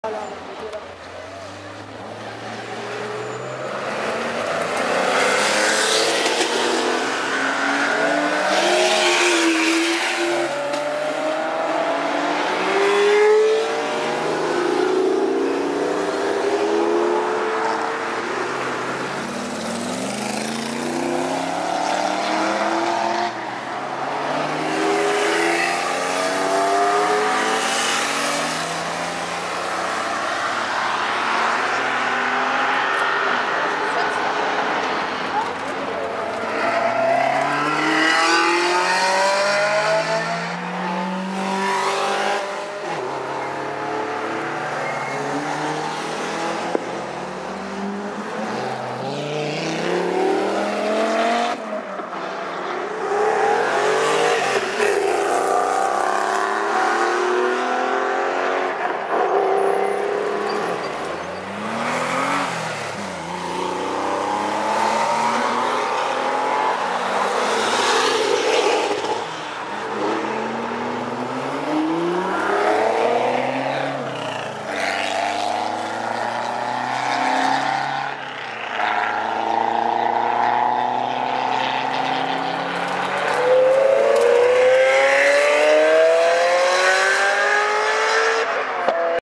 Circuit des Essarts 16/04/2011 part1
Passage des voitures au virage du nouveau monde